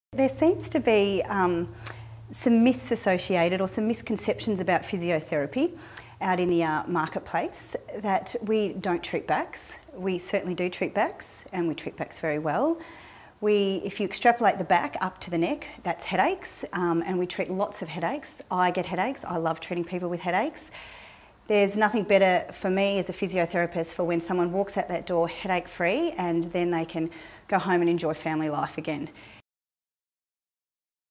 APA Find a Physio Clip 3
Learn about the benefits of physiotherapy and the variety of ways it can help you!